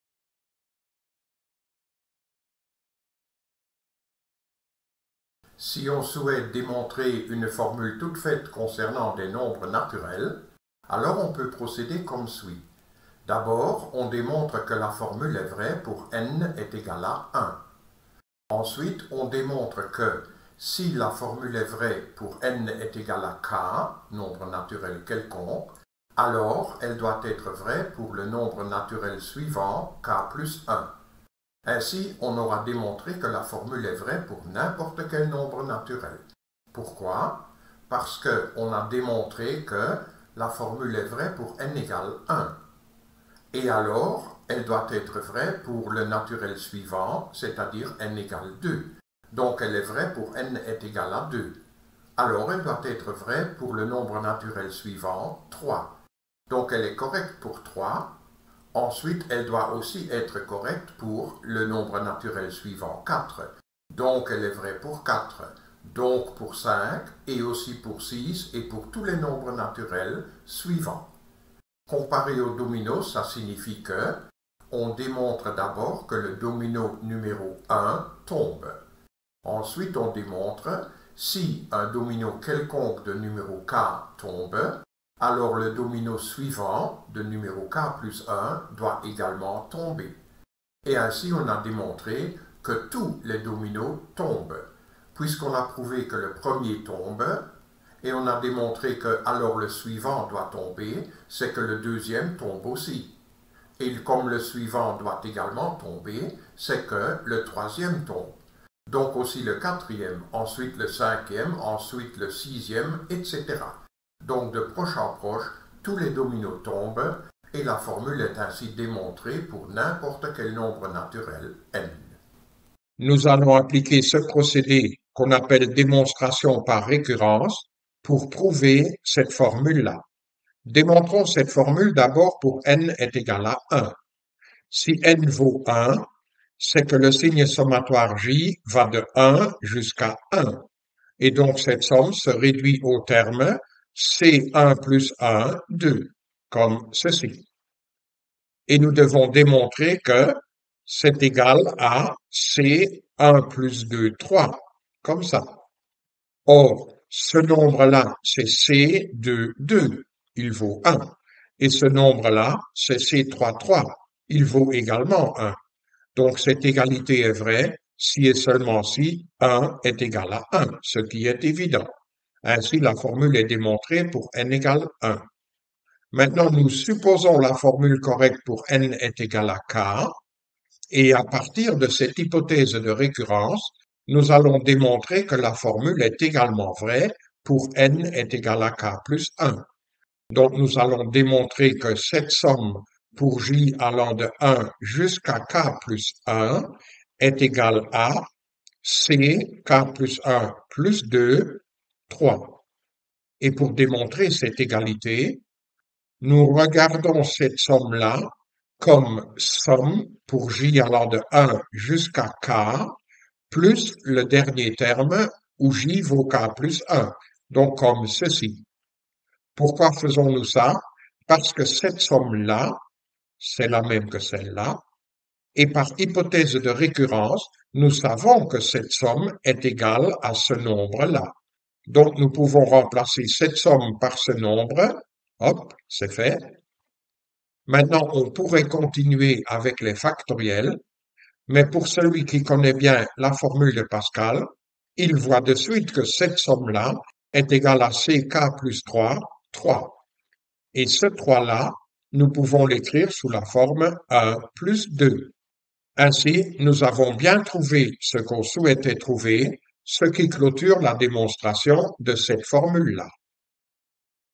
F7013c Démonstration par récurrence
F7013c Nous expliquons d'abord le principe de la démonstration par récurrence en général et ensuite, nous l'appliquons pour démontrer une formule de l'analyse combinatoire, telle qu'elle a été posée lors d'un examen d'admission à l'Université de Liège.